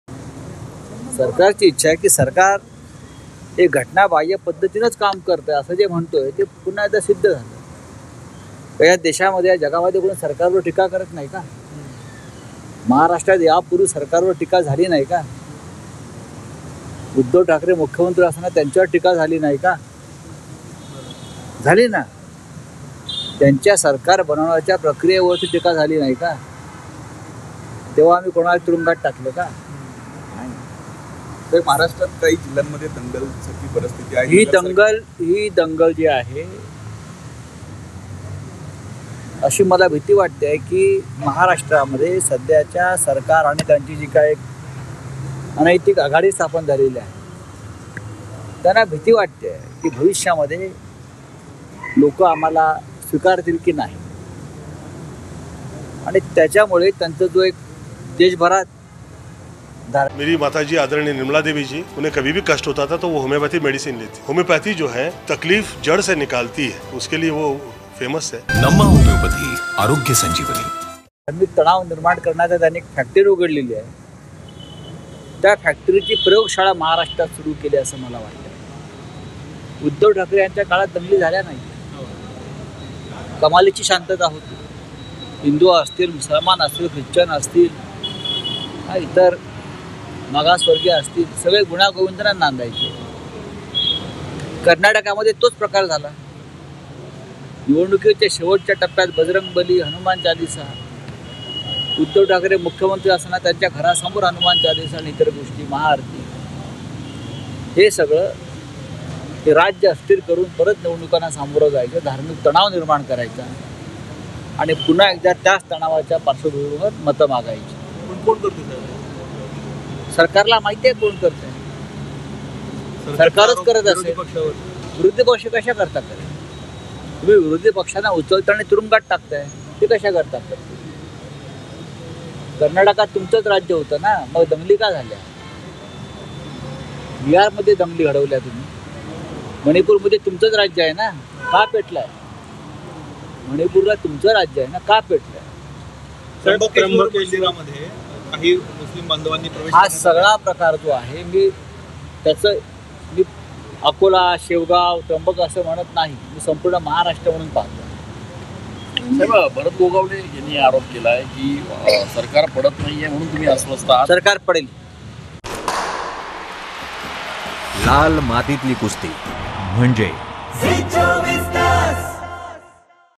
Sanjay Raut On Government | धार्मिक तणाव निर्माण करून मतं मागण्याचा डाव- संजय राऊतांची सरकारवर टीका
16 आमदारांच्या अपात्रतेवरून उपमुख्यमंत्री देवेंद्र फडणवीस यांनी उद्धव ठाकरेंसह महाविकास आघाडीवर निशाणा साधलाय. मविआचा पोपट मेला असल्यां टीकास्त्र त्यांनी सोडलंय. त्यावर फडणवीसांनी असं विधान करावं म्हणजे शहाणपणाची व्याख्या बदलावी लागेल असा टोला राऊतांनी लगावलाय.

Due to the disqualification of 16 MLAs, Deputy Chief Minister Devendra Fadnavis has targeted Mahavikas Aghadi along with Uddhav Thackeray. He left a comment saying that Mavia's parrot is dead. Raut has threatened that Fadnavis should make such a statement that the definition of wisdom will have to be changed.

Social Media Links :
5 Minitat 25 Batmya । 5 मिनिटांत 25 बातम्या